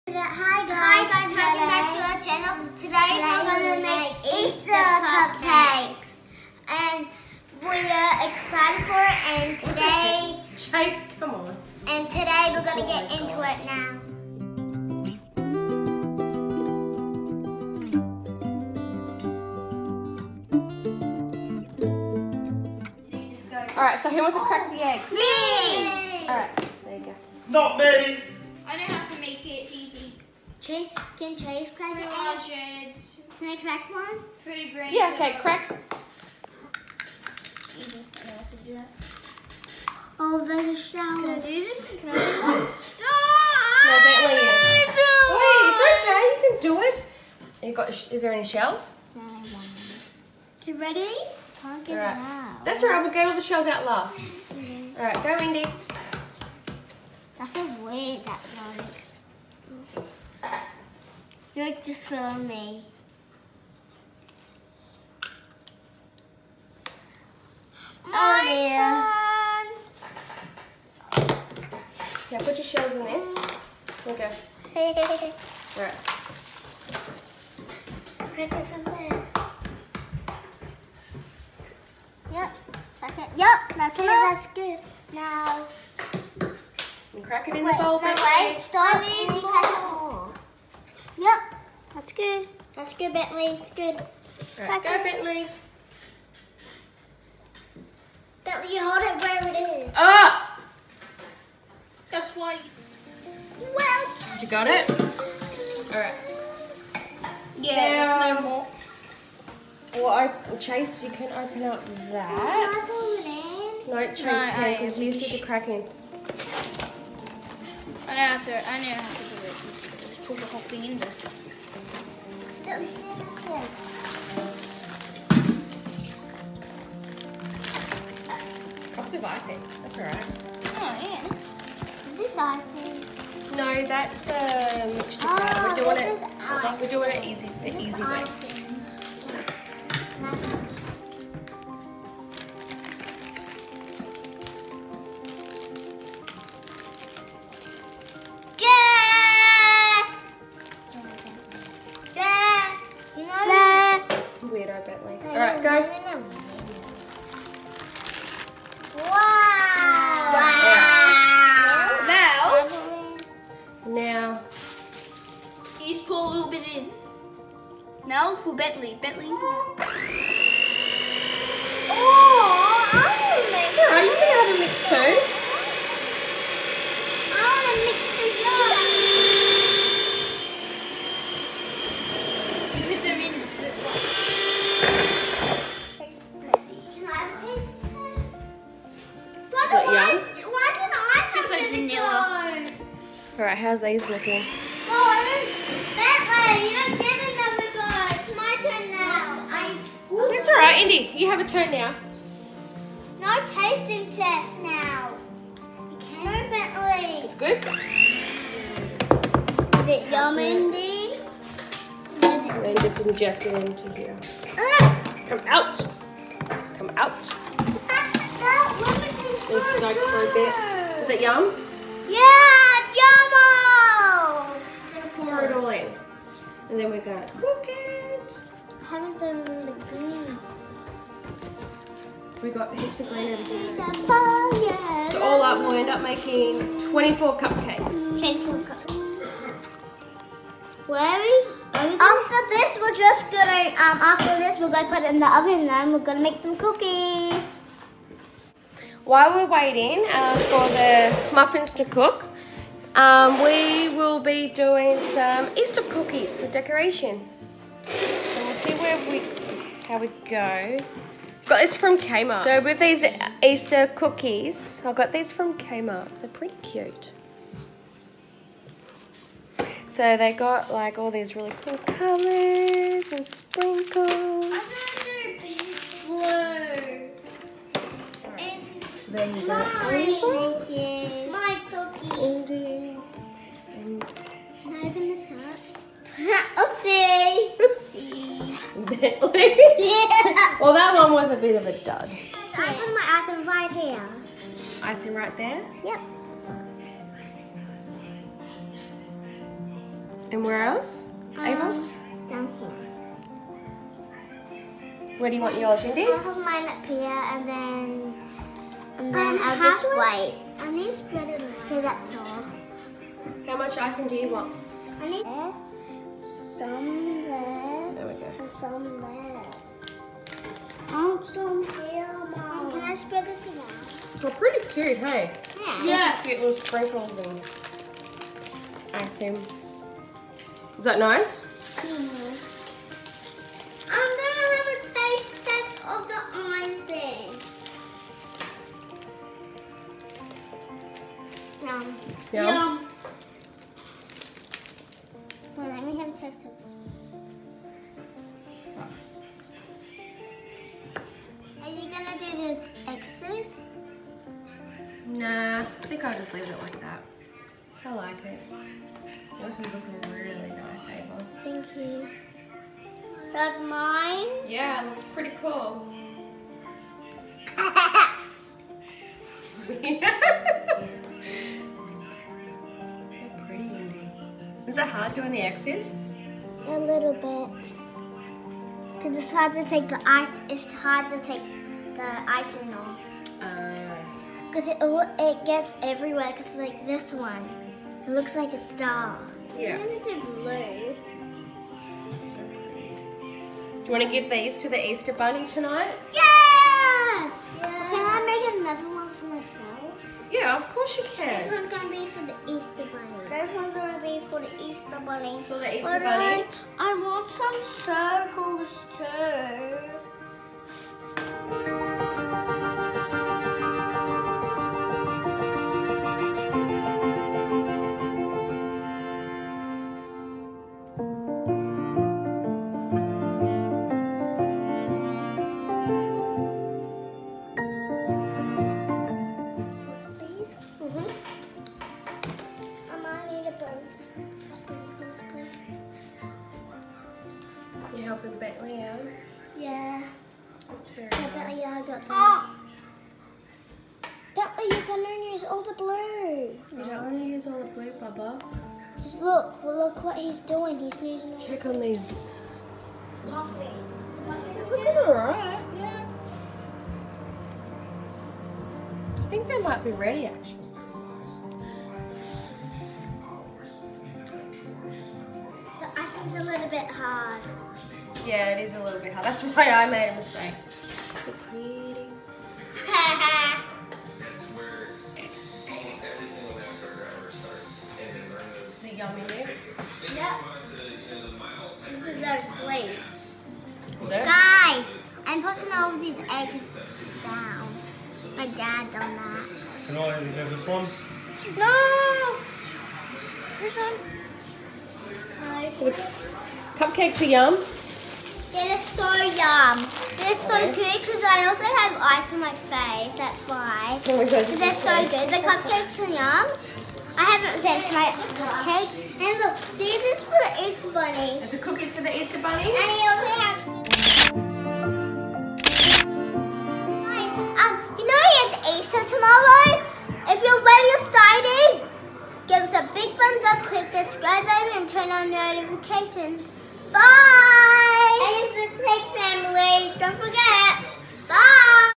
Making EASTER cupcakes and cookies
In this video we make Easter cupcakes and Easter cookies with the tegg family